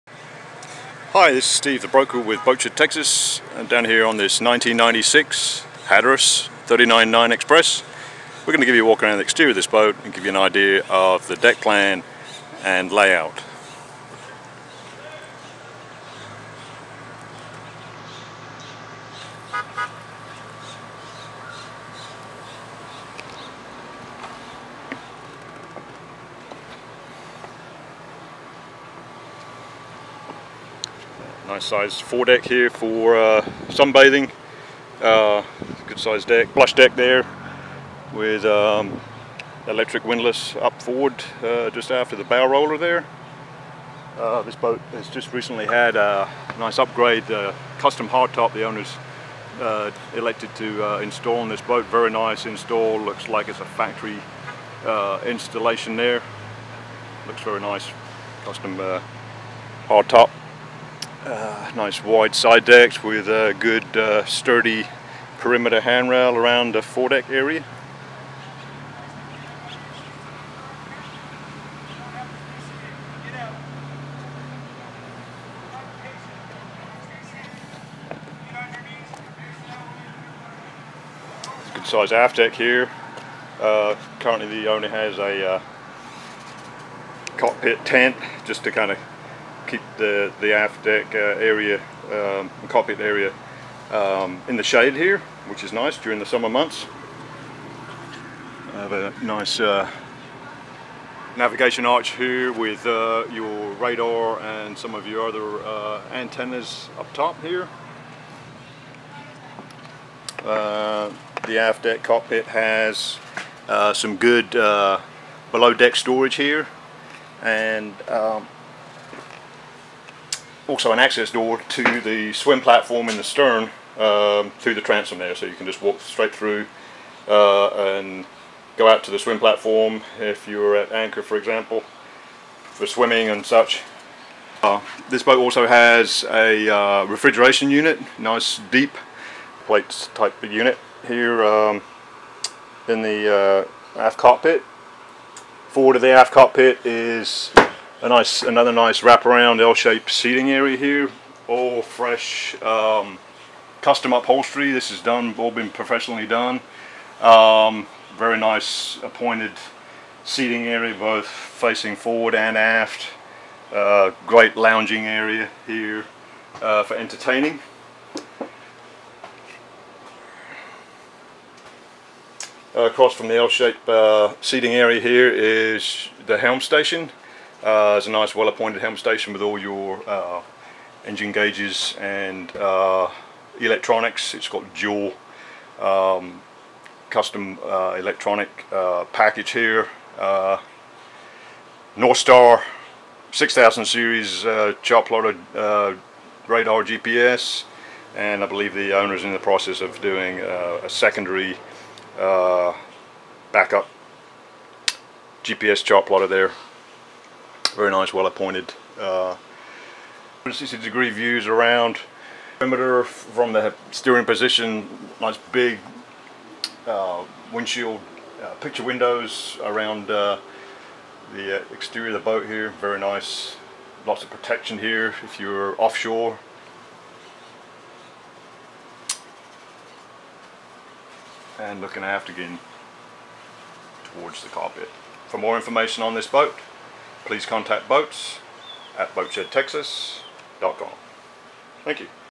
Hatteras 39.9 Express Interior Refit_Near Completion - Boatshed - Boat Ref#303153
Hatteras 39.9 Express Interior Refit_Near Completion for sale with Boatshed Texas Photos and video taken by Boatshed Texas